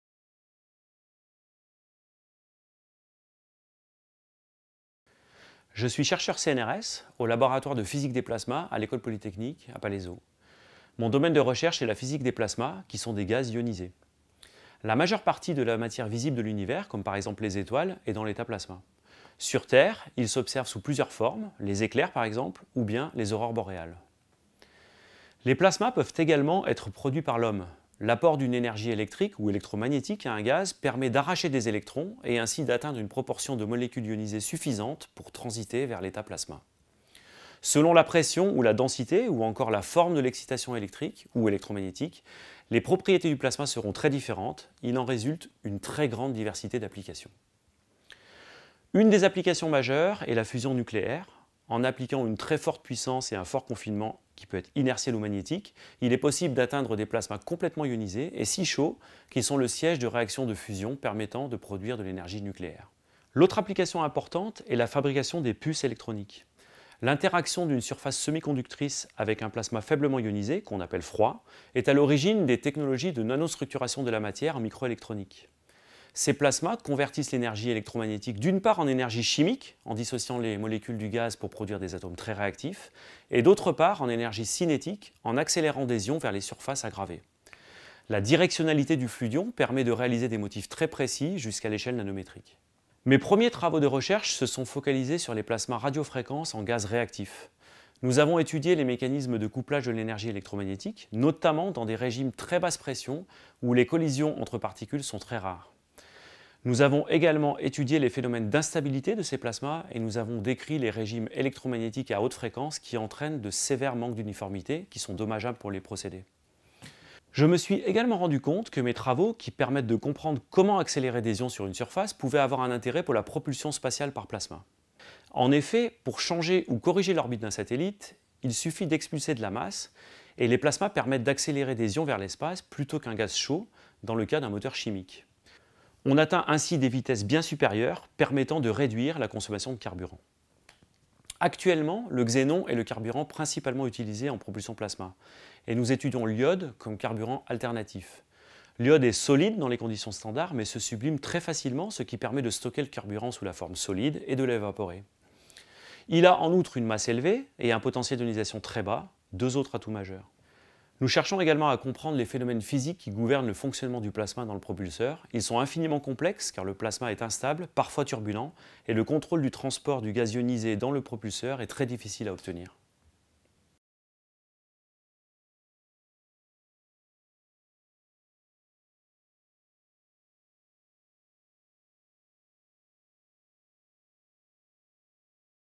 Pascal Chabert, directeur de recherche en physique des plasmas | Talents CNRS
Pascal Chabert, directeur de recherche CNRS au Laboratoire de physique des plasmas (LPP - CNRS/École polytechnique/Sorbonne université/Observatoire de Paris/Université Paris-Saclay), a reçu la médaille d’argent 2022 du CNRS.

Depuis sa thèse, Pascal Chabert a pour ambition de maîtriser et d’exploiter les propriétés des plasmas, des états de la matière à très haute température notamment présent dans la couronne solaire. De Berkeley aux CNRS, le chercheur s’intéresse en particulier aux plasmas froids, qui sont produits en laboratoire à des fins industrielles. Il s’est tout d’abord intéressé aux plasmas radiofréquence utilisés en microélectronique puis s’est consacré par la suite à la propulsion spatiale par plasma. Pascal Chabert est ainsi à l’origine d’un concept breveté proposant l’iode comme fluide de propulsion. Il collabore également avec Safran pour le développement d’outils de simulation afin d’améliorer les performances et la durée de vie de propulseurs plasmiques dit à « effet hall ». Ses recherches s’inscrivent ainsi au cœur de la course au développement de la future génération de moteurs spatiaux.

La médaille d'argent distingue des chercheurs et des chercheuses pour l'originalité, la qualité et l'importance de leurs travaux, reconnus sur le plan national et international.